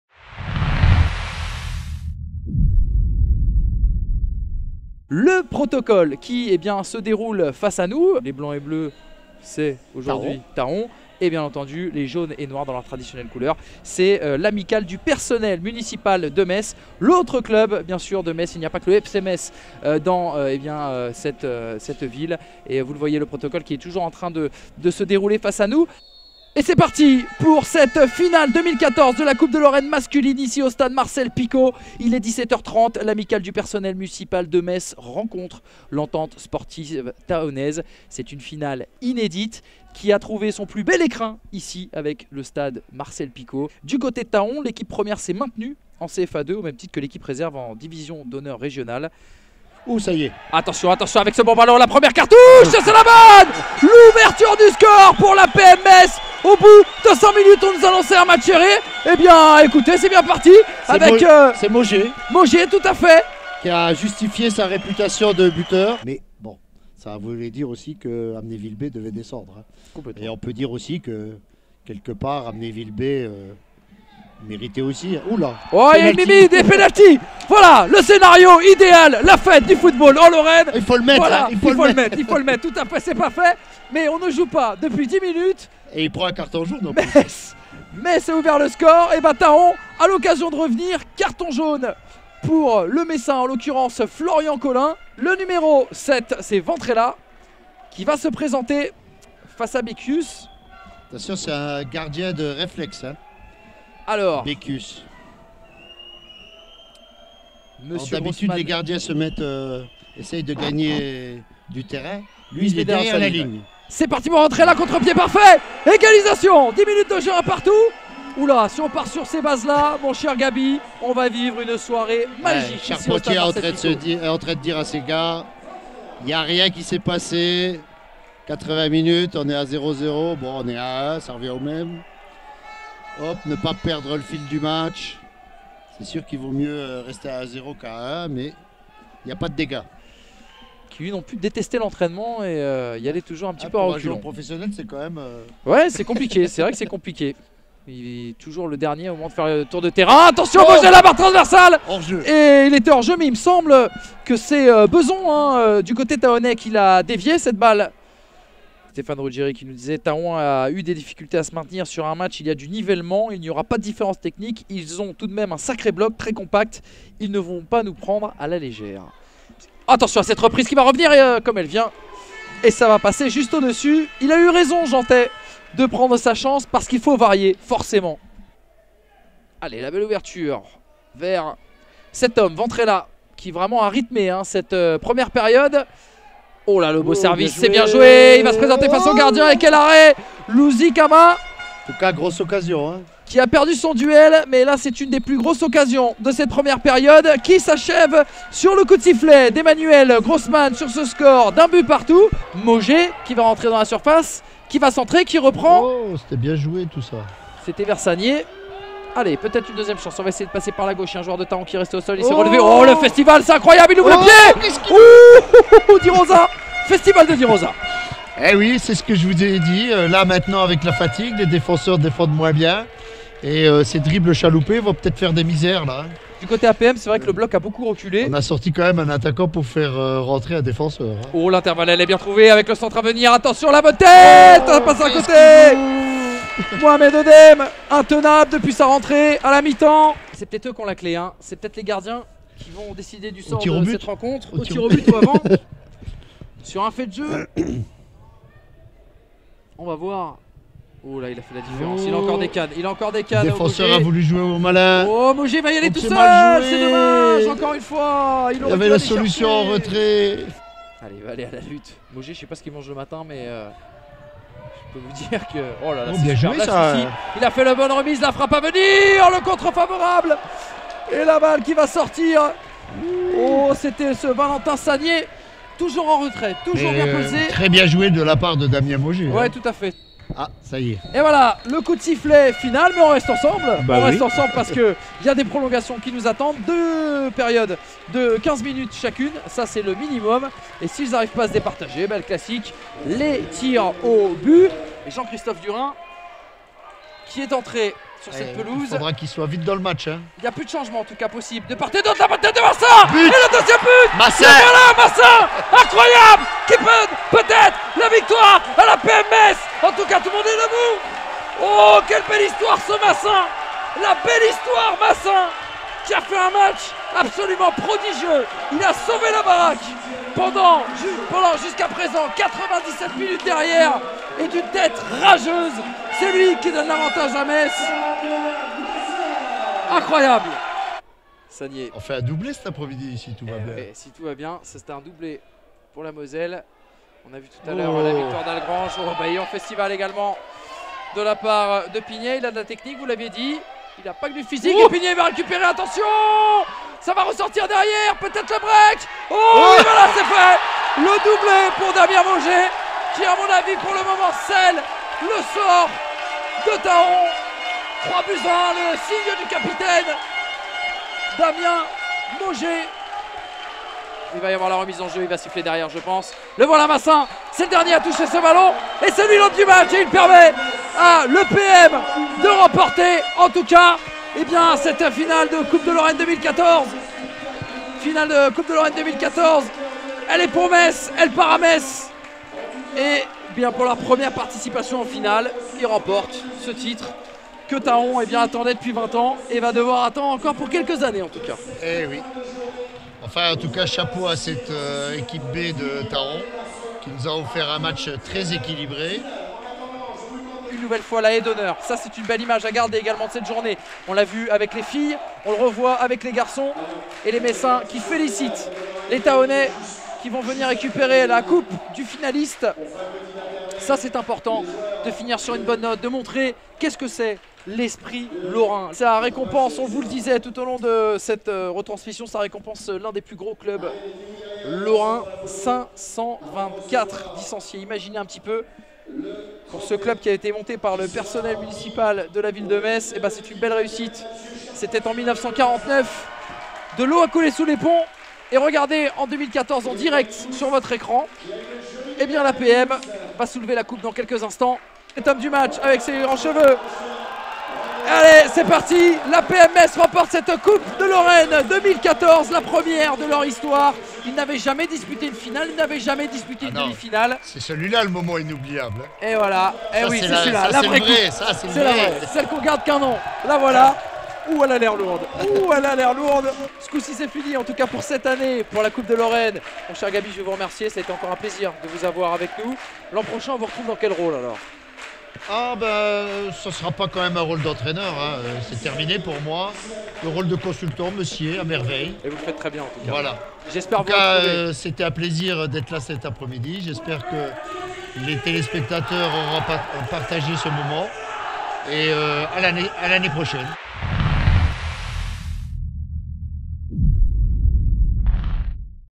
Finale Coupe de Lorraine : APM Metz 3 - 1 Thaon (07/06/2014)
APM Metz 3 - 1 Thaon(b) (07/06/2014)

Buts pour Metz : Damien MAUGER (6' ; 107') ; MASSING (98')
But pour Thaon : Nolan VENTRELLA (10')

Finale Coupe de Lorraine 2014